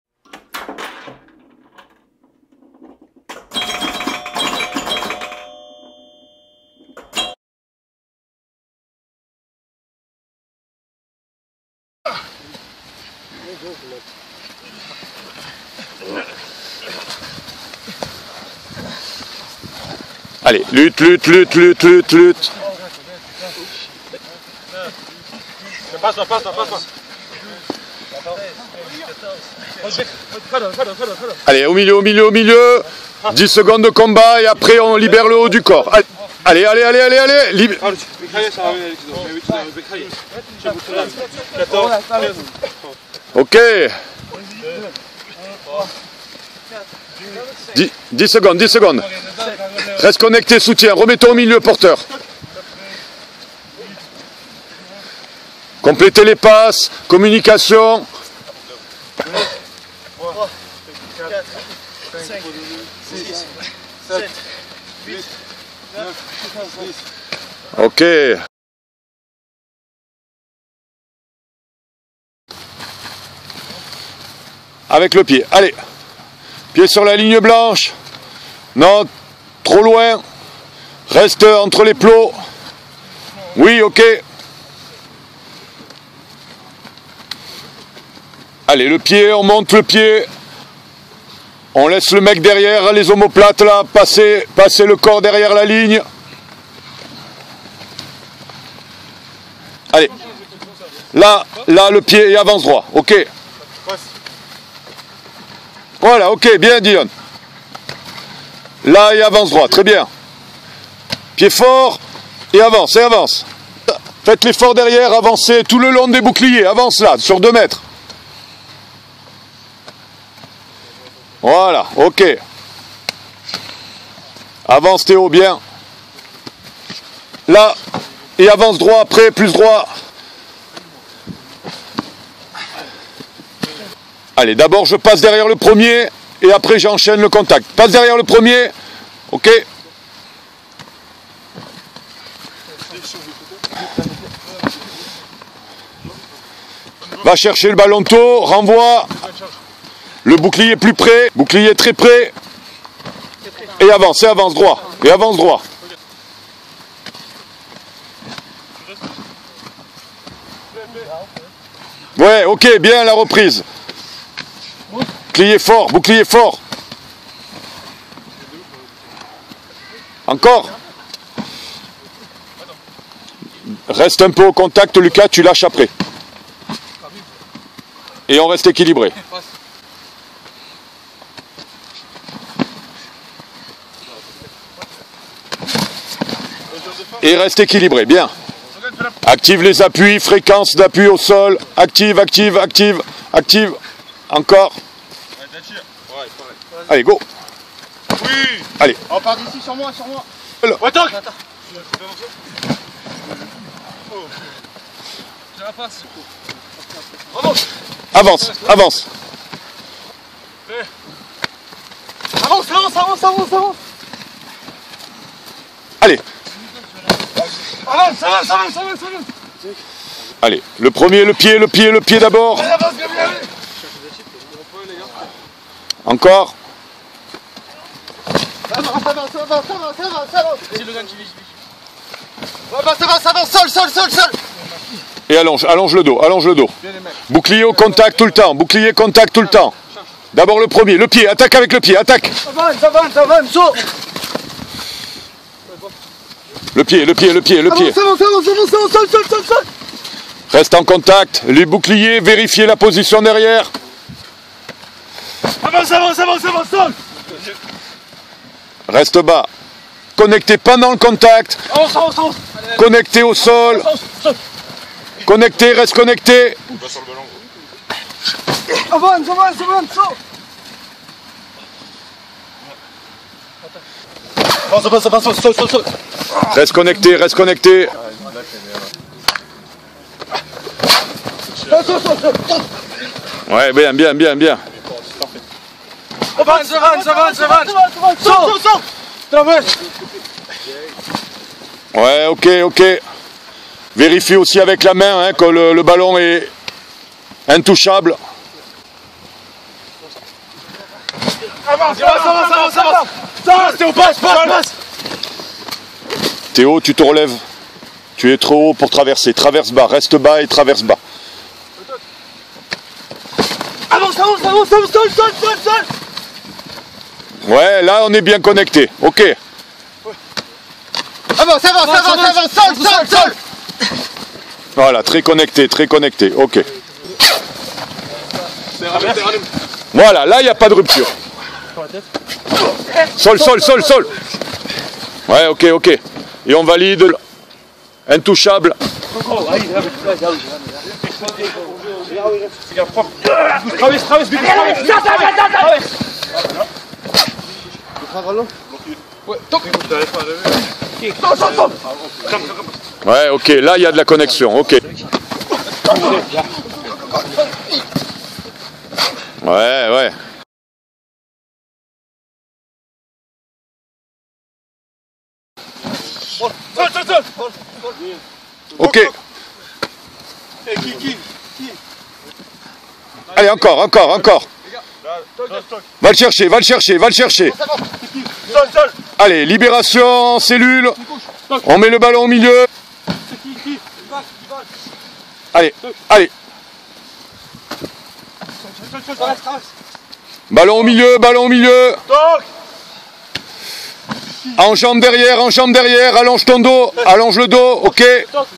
TECHNIQUE INDIVIDUELLE ROUTINES SOL/RUCK OFFENSIF : CONSERVATION SITUATION FAVORABLE
Technique individuelle Ruck et conservation.
Continuité et jeu debout avant toute chose bien sur.
La qualité du contact génère la situation favorable au sol.
Focus sur l'intervention le plus tôt possible (debout) des soutiens et sur la triple connexion (dans la règle) : Porteur responsable du ballon + 1er Soutien sur 1ère menace + 2 ème soutien valide ou utilise.